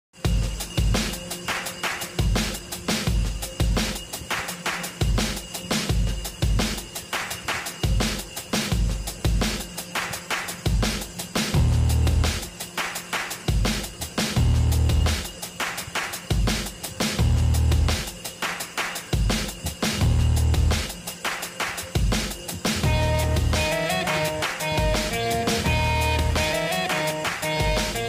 Mobile with handle and beautiful cover
Take cellphen paper or transparent sheet cut it according to the ur mobile size and take pics of the cloth , stick with cello tape and make handle with it.stick it at side of the mobile where no buttons. thanks